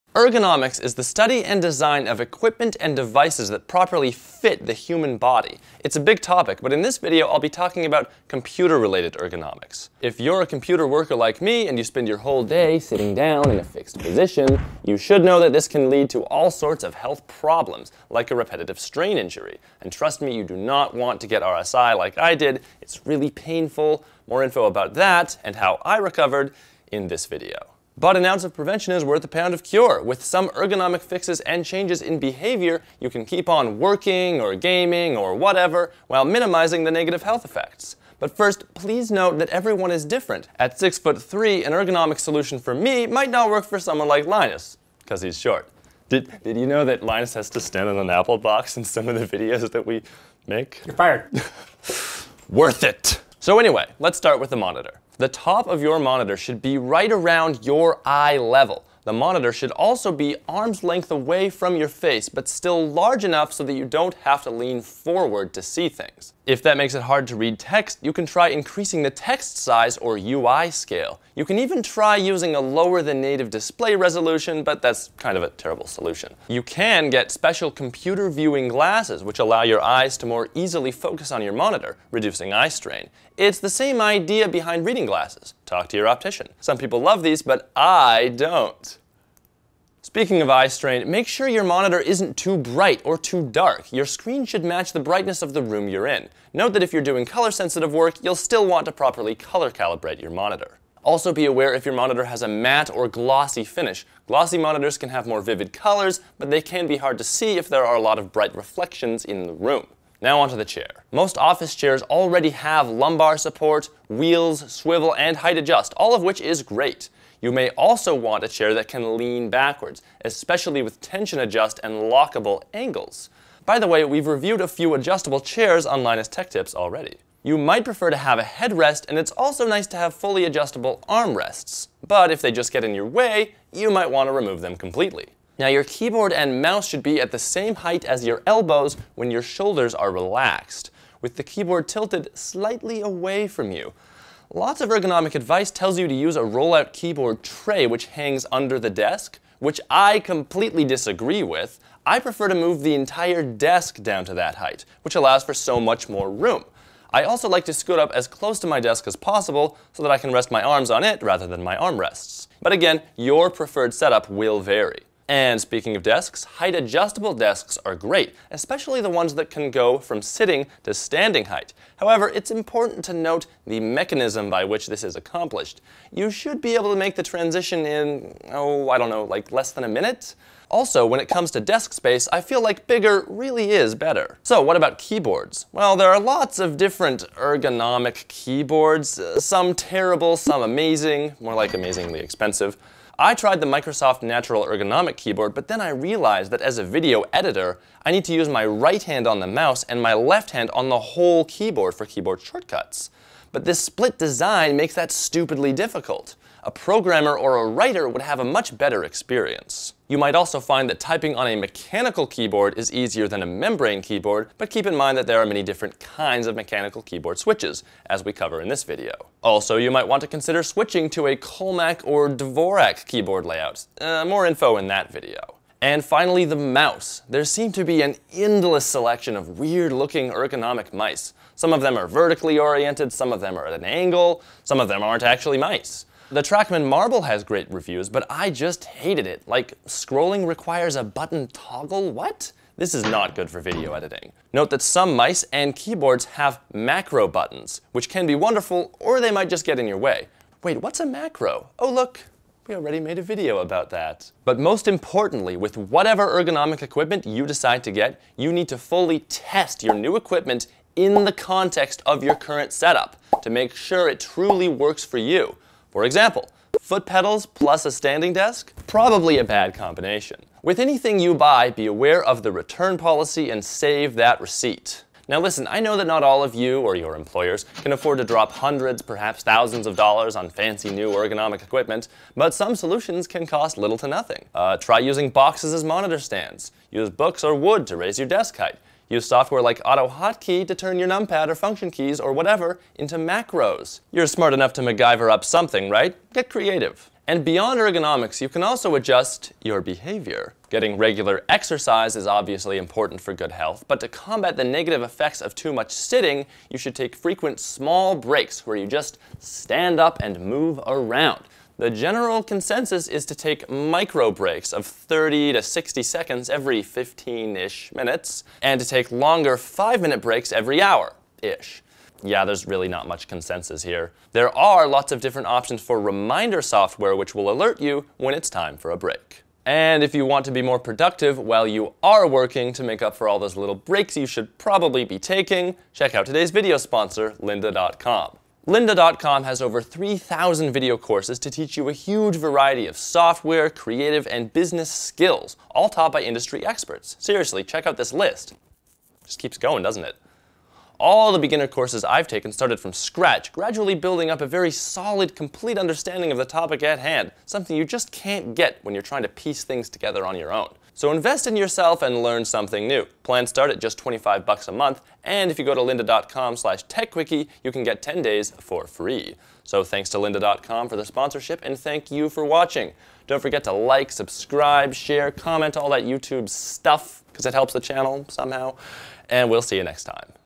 Computer Ergonomics as Fast As Possible
Sitting at a computer all day can lead to all sorts of health problems! Taran explains different ergonomic and behavior solutions.

Sponsor message: lynda.com is your one stop shop for learning a variety of skills online, at your own pace.